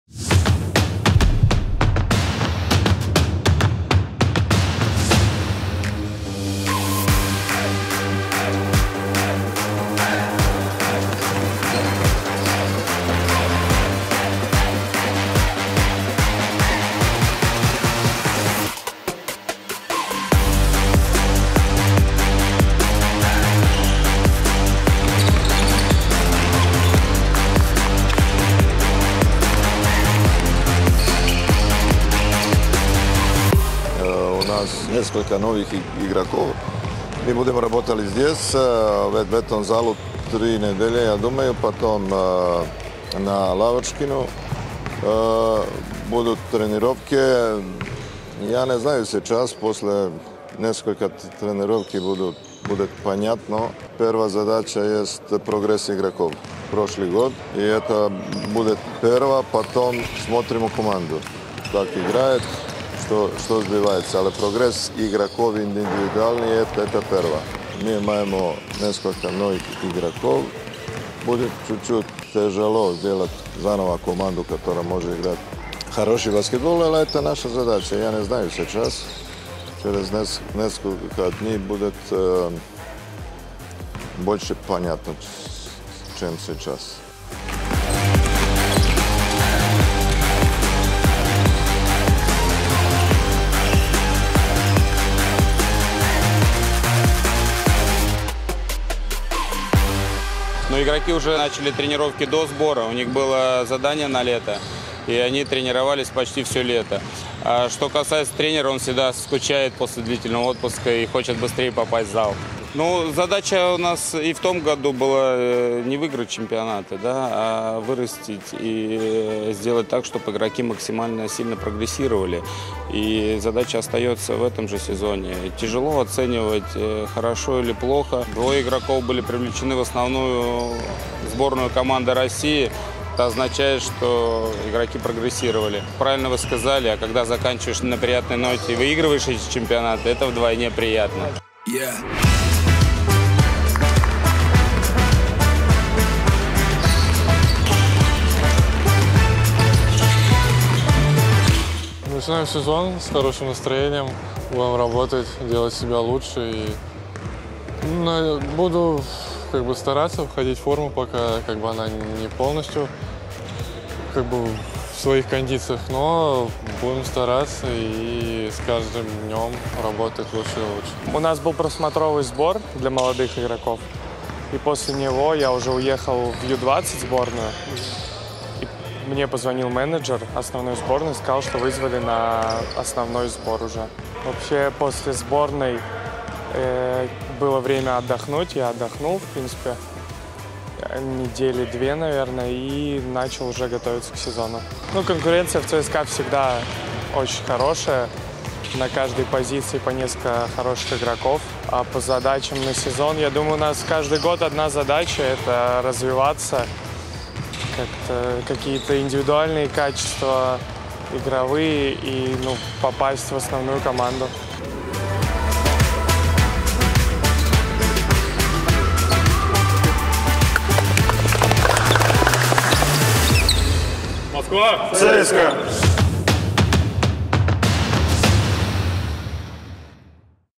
CSKA-2 starts the preseason / ЦСКА-2 начинает подготовку к сезону
Репортаж CSKAbasketTV о начале подготовки армейского дубля. Серьёзные тренировки с самого начала для выполнения самых серьёзных задач.